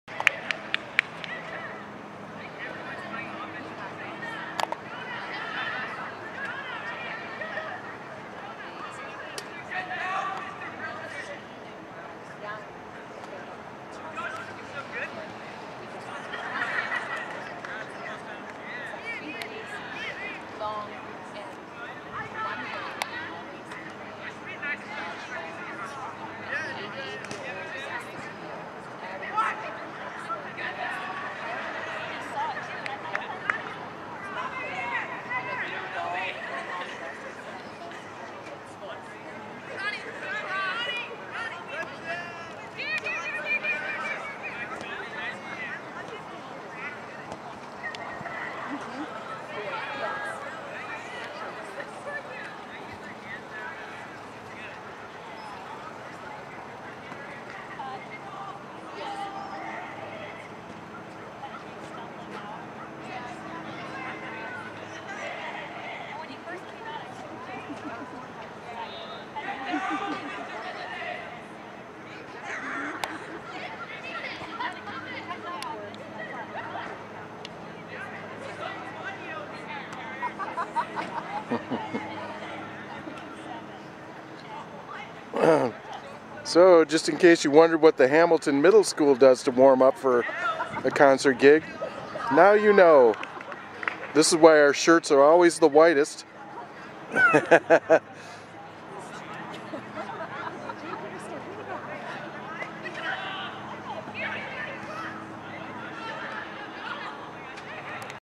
HIMS Senior Jazz Relaxes before the University of Portland Jazz Festival April, 2014
Senior Jazz plays a bit of soccer prior to playing their set. Extra points for no grass stains!